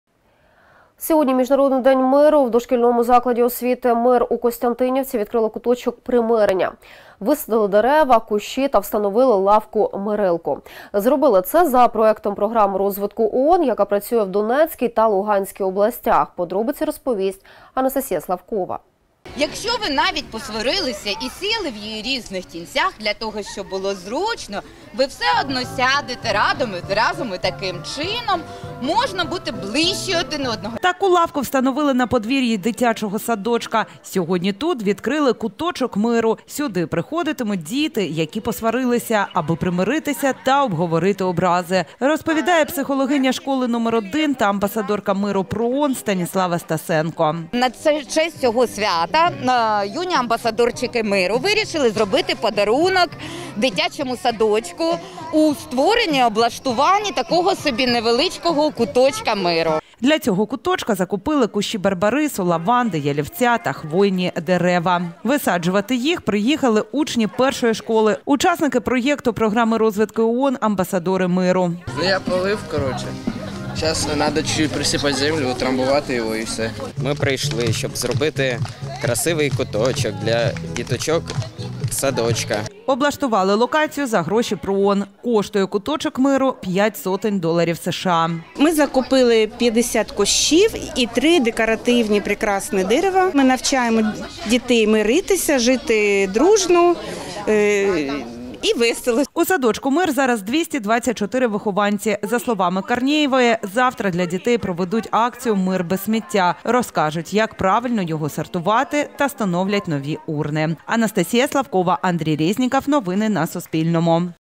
У Костянтинівці відкрили куточок примирення
Сьогодні в Міжнародний день миру в дошкільному закладі освіти «Мир» у Костянтинівці відкрили куточок примирення – висадили дерева, кущі та встановили лавку-мирилку. Зробили це за проектом програми розвитку ООН, яка працює в Донецькій та Луганській областях.

Будь в курсі подій регіону з Суспільним